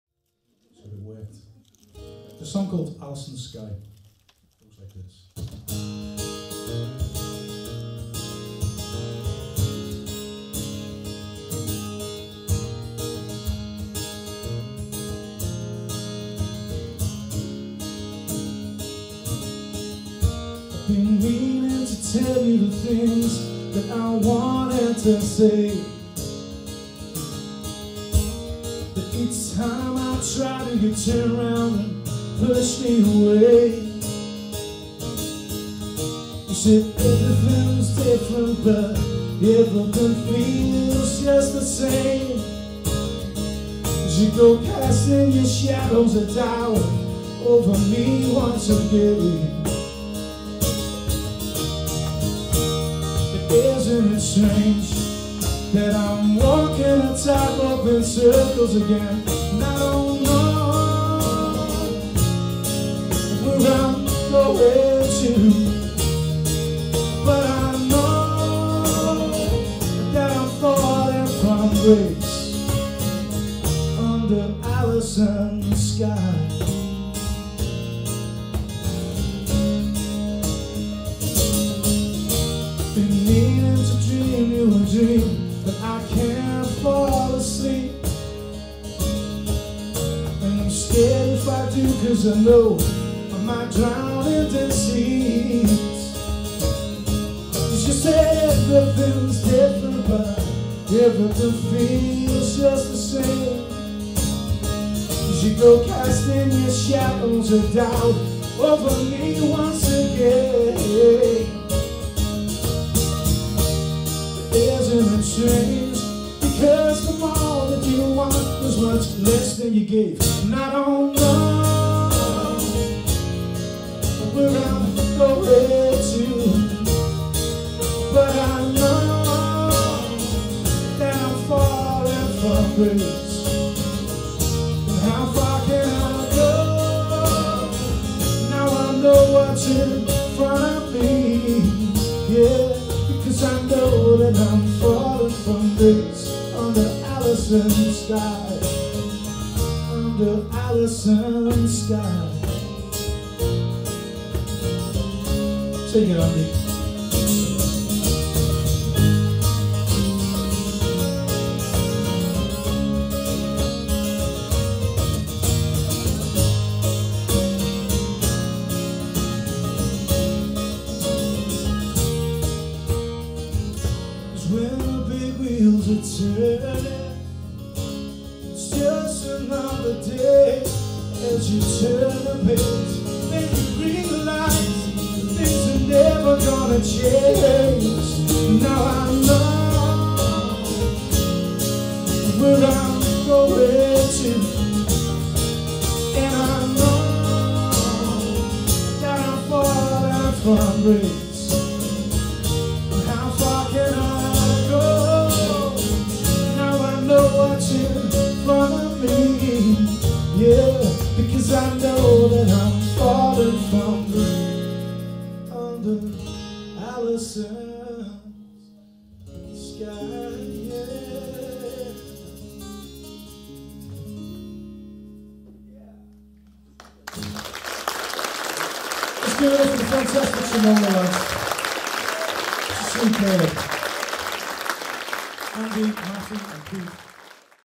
Alison's Sky BW
Aliso's Sky recorded live at the A5OO club Stoke on Trent